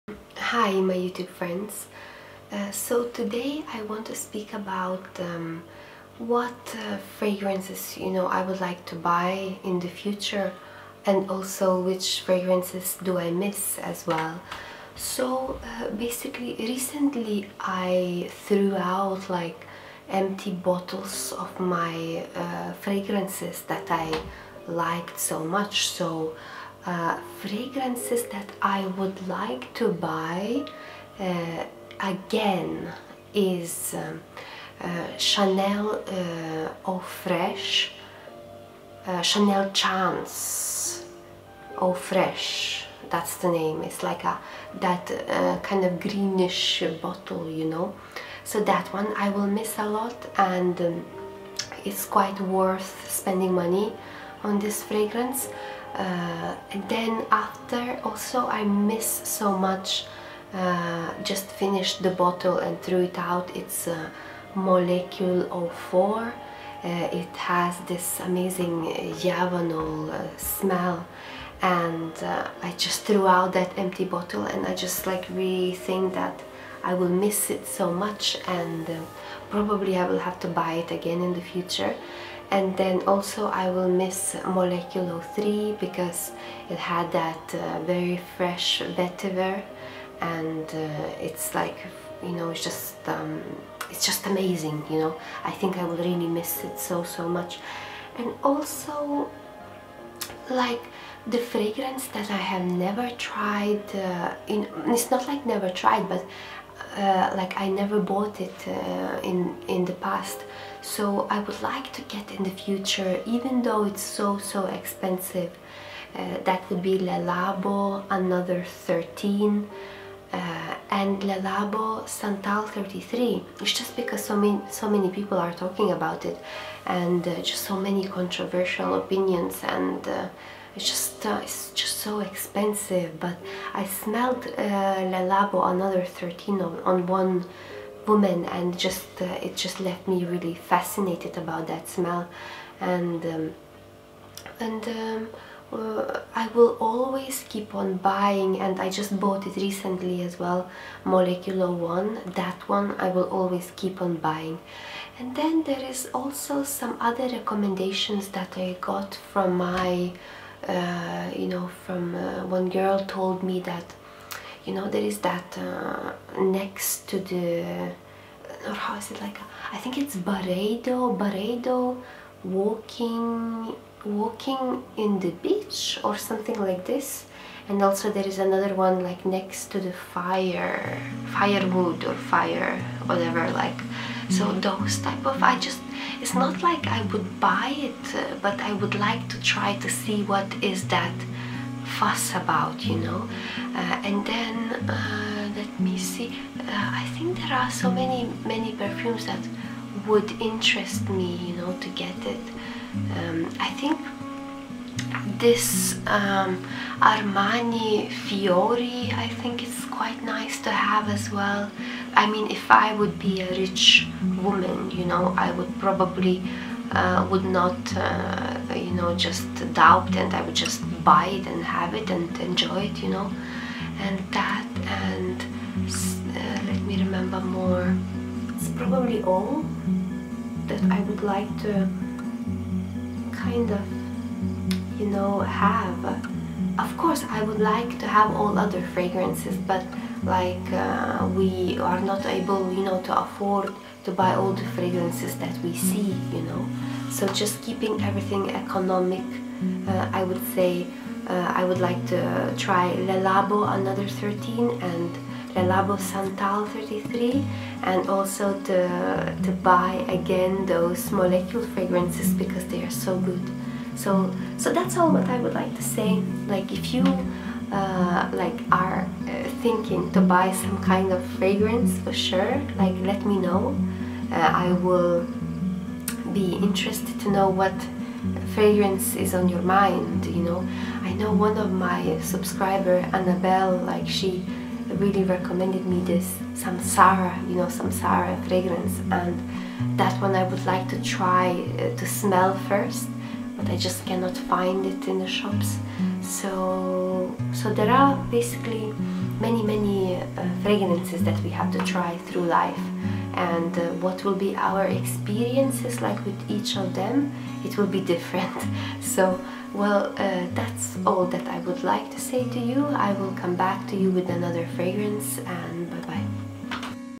🌼FRAGRANCES I AM DREAMING TO GET IN THE FUTURE 👌
Hi ,

Which fragrances i am hoping to get in the future .
There are just few of them . Tell me if any of them would be on your list as well 😆?Which one is on your list to buy fragrances ?
Let's dream about it more😊.

Music used  in this video is owned by me .